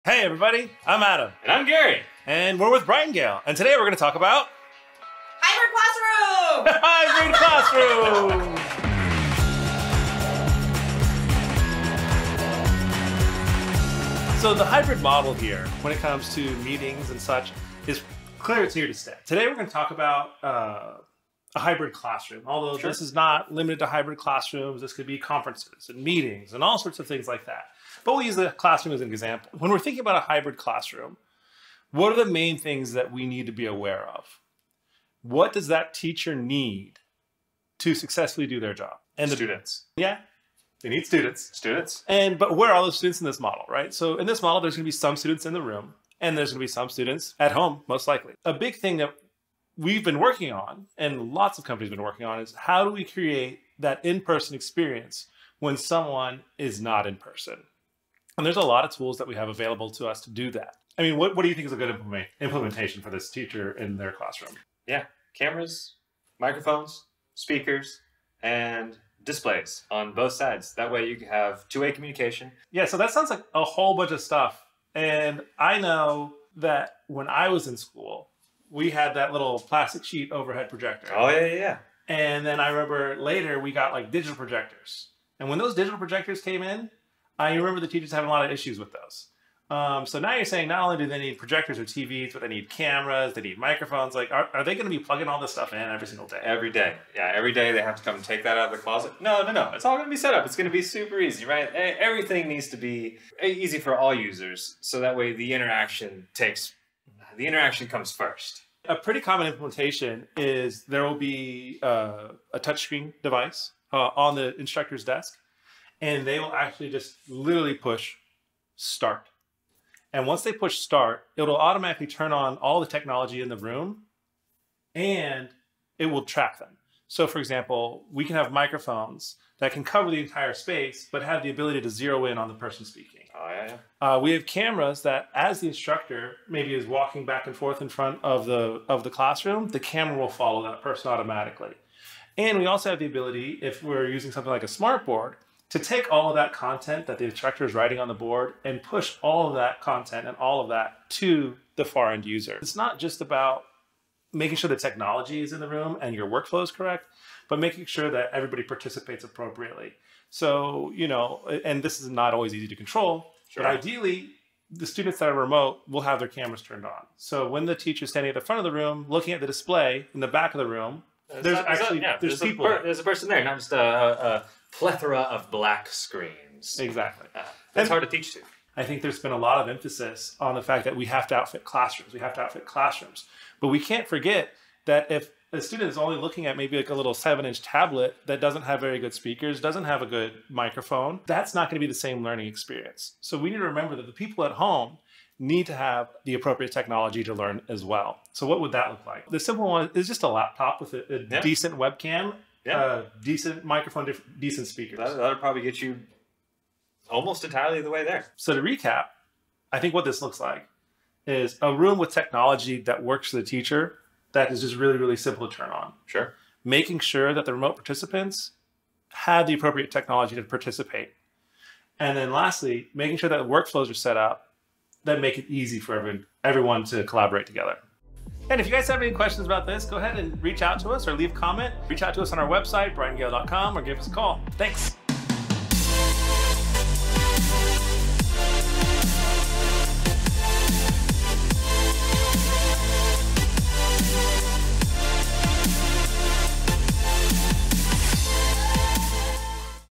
Technology for Hybrid Classrooms – How-To Prepare for Success
The hybrid model is here to stay. Brightengale's Adam Keho and Gary Bailey share a few ways to prepare your classrooms with the right technology to create seamless experiences for teachers and students alike.

Frame • Network Infrastructure
Amplify • Audio Visual
Support • Systems & IT
Protect • Security & Access

All opinions in the video are our own. Brightengale did not receive any compensation for our review.

Brightengale | Tech on a High Note
Based out of Houston, TX